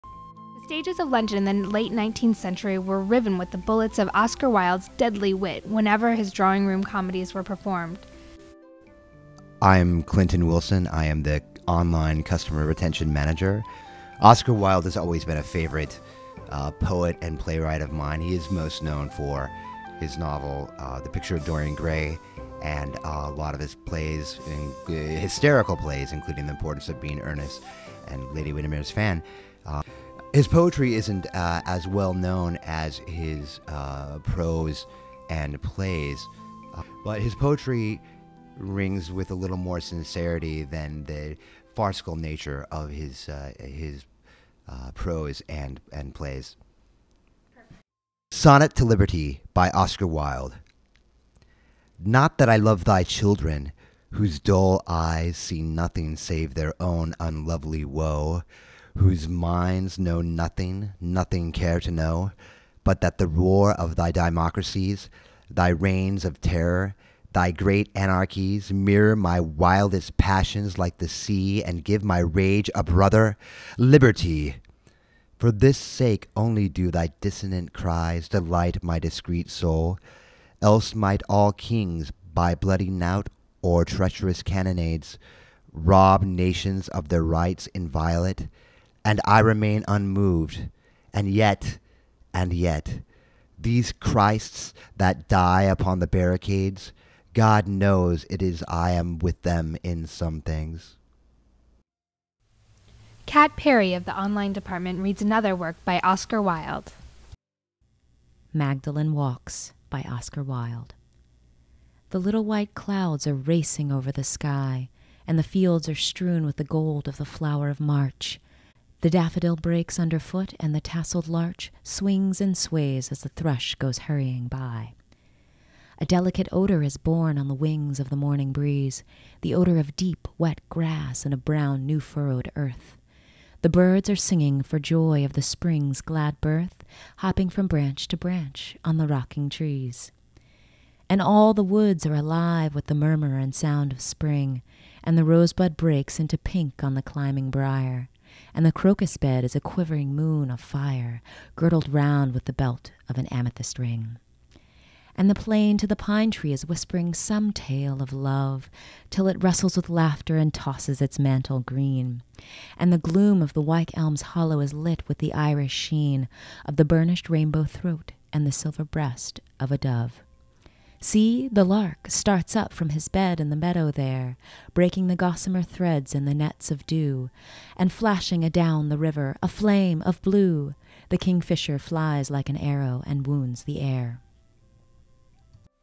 A Cup of Poetry: The Luck of the Irish
Part 2 - Oscar Wilde

Clinton Wilson reads "Sonnet to Liberty" and Cat Perry reads "Magdalen Walks"